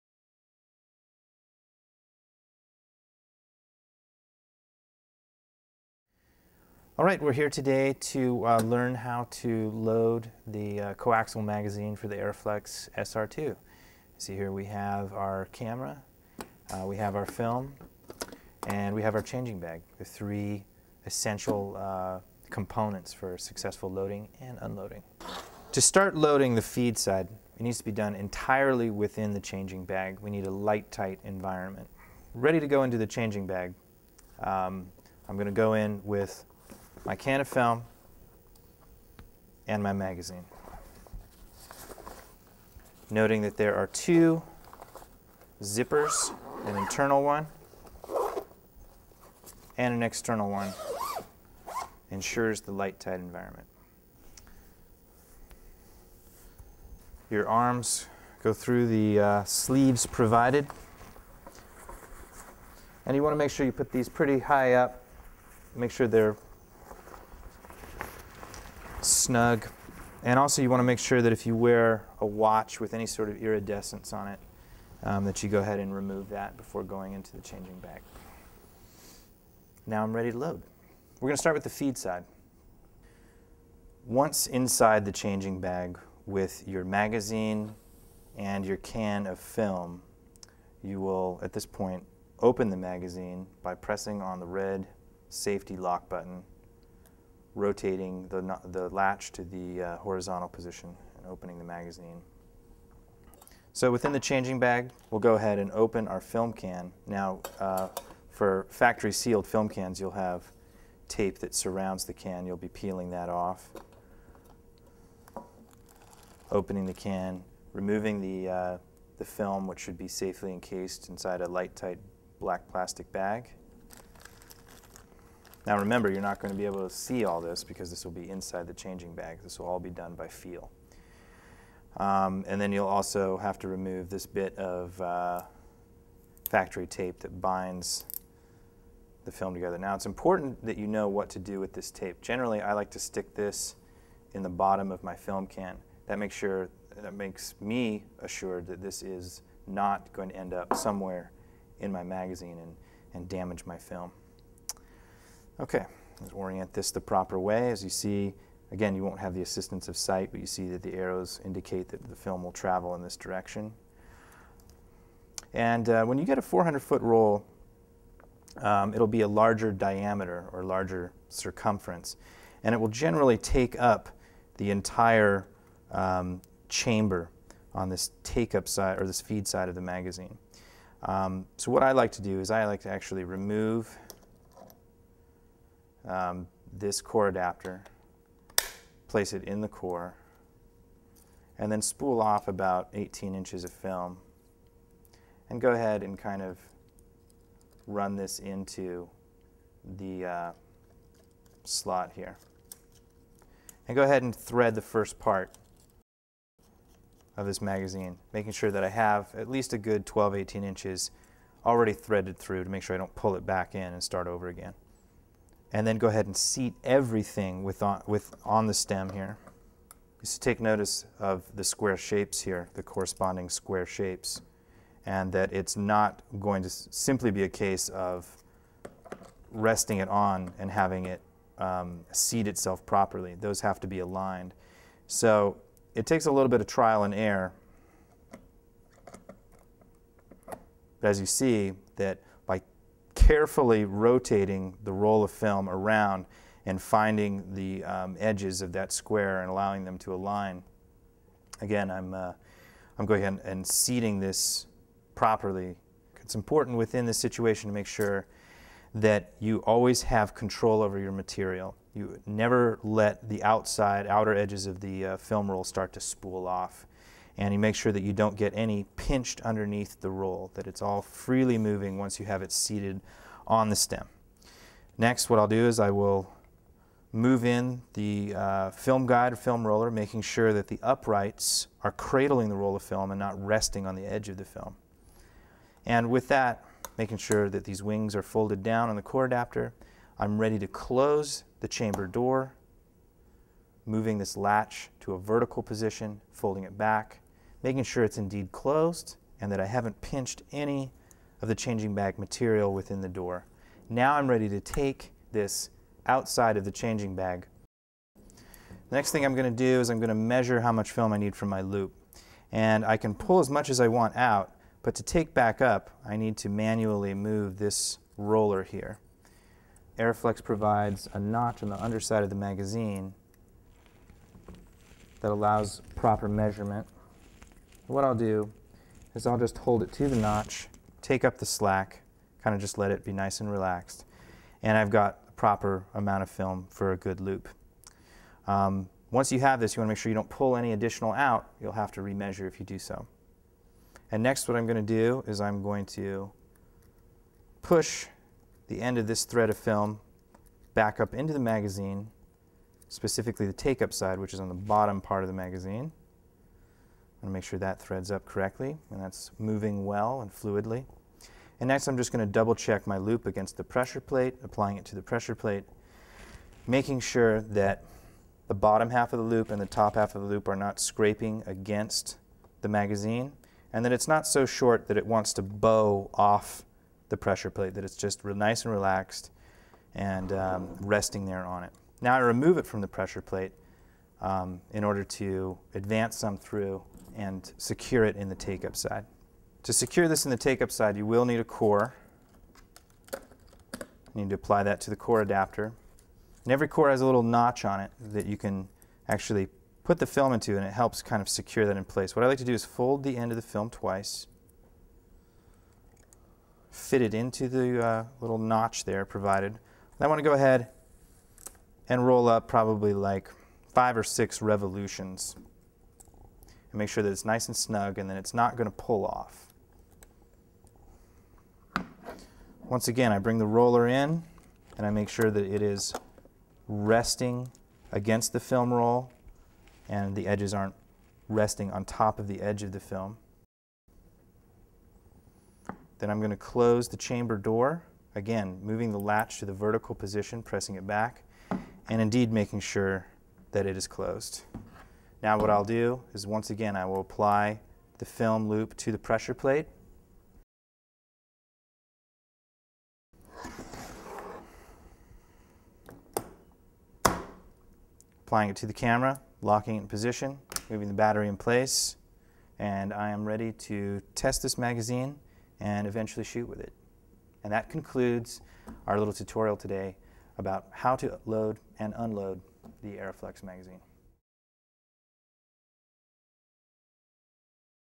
Loading the Arriflex SR2 Magazine
Professor Plow demonstrates how to load the Arriflex SR2 mag